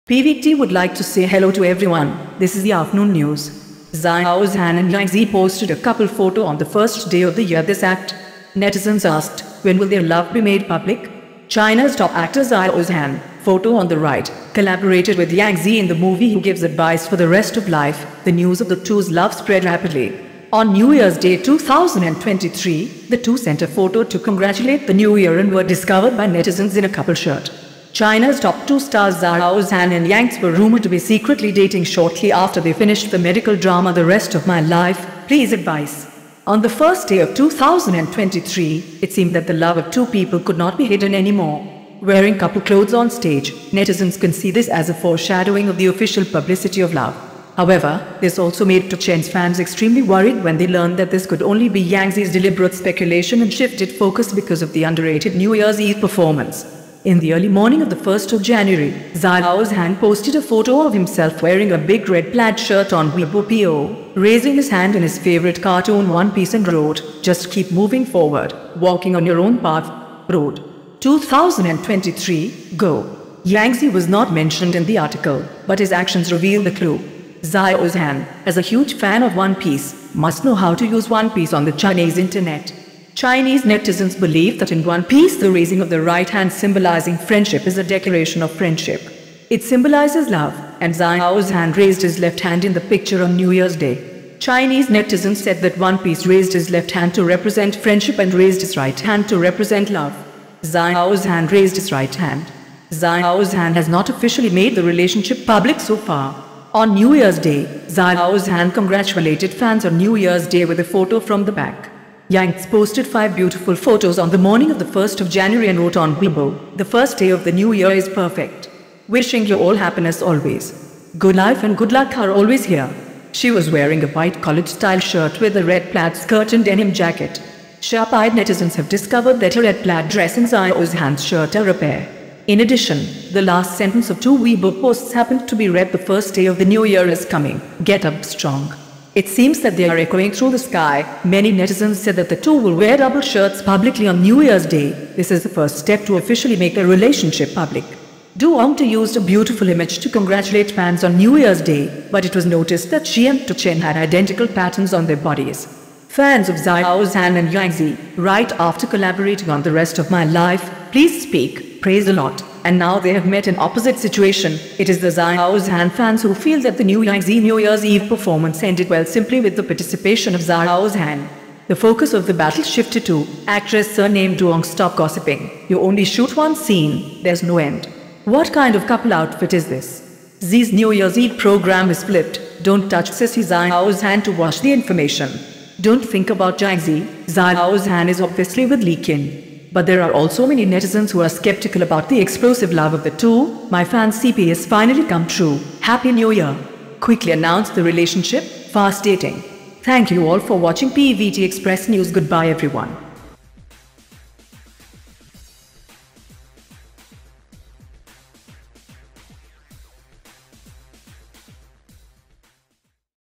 Xiao Zhan and Yang Zi posted couple outfits on New Year's Day, when will it be officially announced
Xiao Zhan and Yang Zi posted a couple photo on the first day of the year "This act." Netizens asked: When will their love be made public? China's top actor Xiao Zhan (photo on the right) collaborated with Yang Zi in the movie "Who gives advice for the rest of life", the news of the two's love spread rapidly. On New Year's Day 2023, the two sent a photo to congratulate the new year and were discovered by netizens in a couple shirt.
China's top two stars Xiao Zhan and Yangtze were rumored to be secretly dating shortly after they finished the medical drama The Rest of My Life, Please Advice. On the first day of 2023, it seemed that the love of two people could not be hidden anymore. Wearing couple clothes on stage, netizens can see this as a foreshadowing of the official publicity of love. However, this also made Tieu Chien's fans extremely worried when they learned that this could only be Yangzi's deliberate speculation and shifted focus because of the underrated New Year's Eve performance.